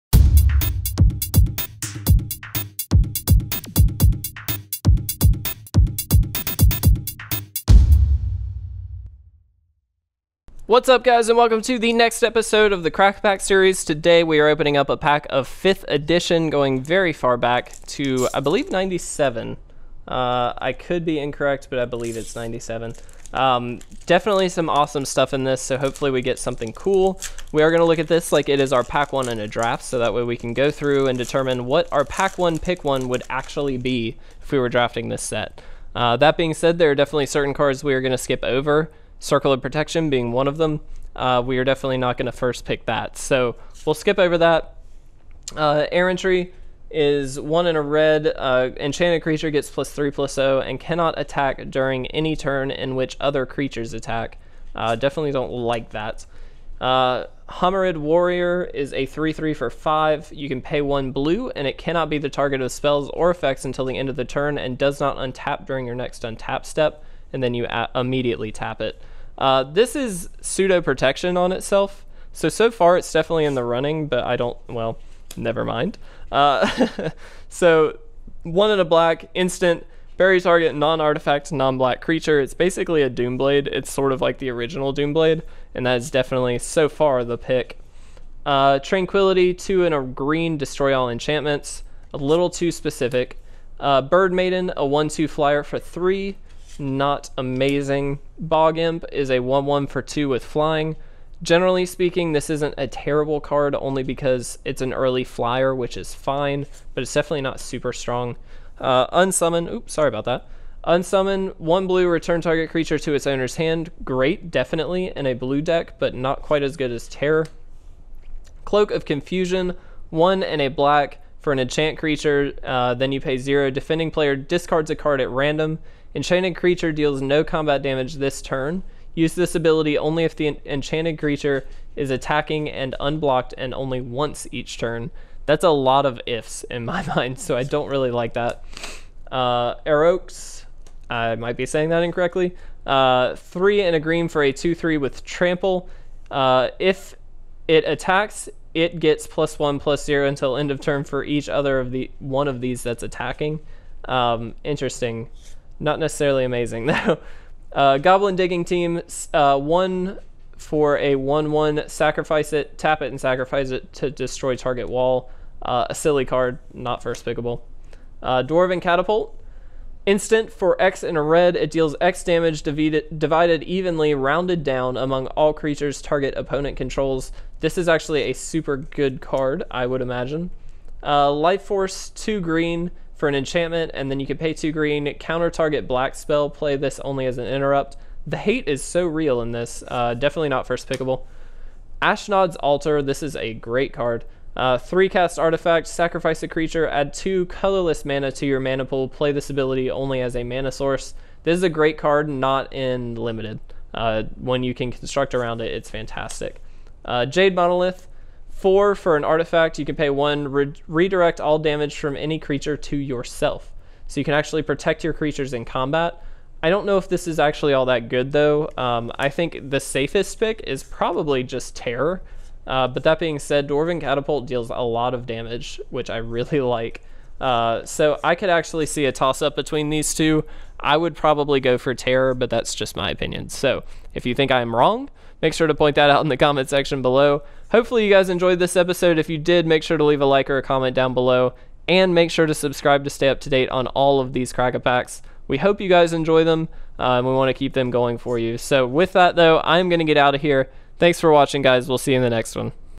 Crack a Pack ep133 5th Edition
Today we open up a pack of 5th Edition! If you enjoy this video, make sure to like or leave a comment to show your support!